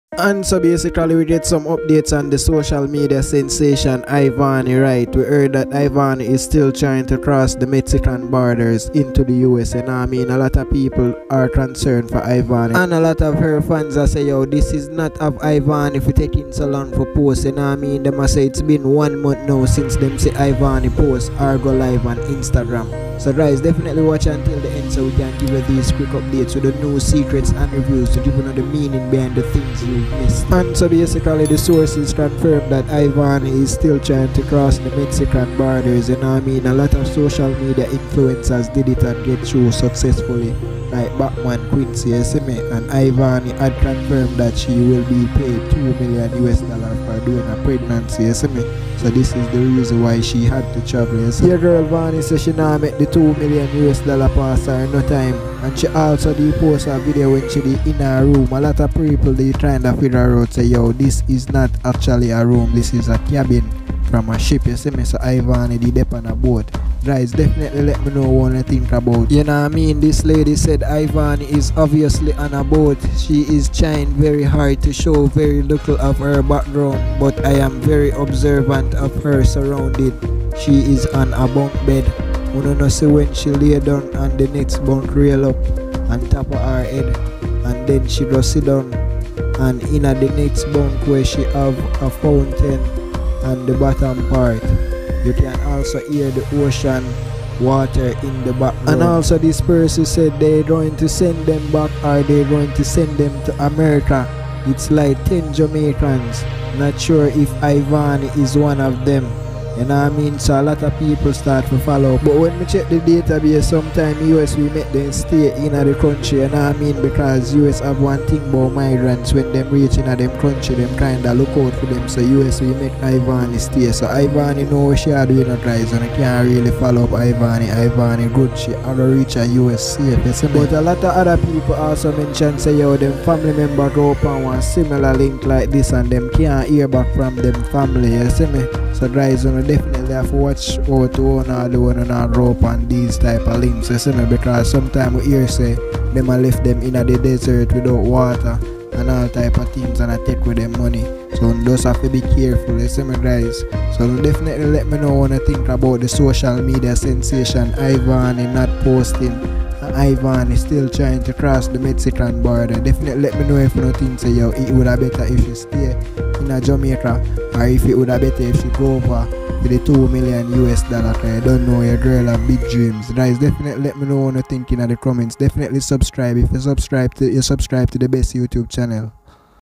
Ivany Still Tryna Cross The Border 💔
Ivany Still Tryna Cross The Border Proof In This Video.

JAMAICA SHADE ROOM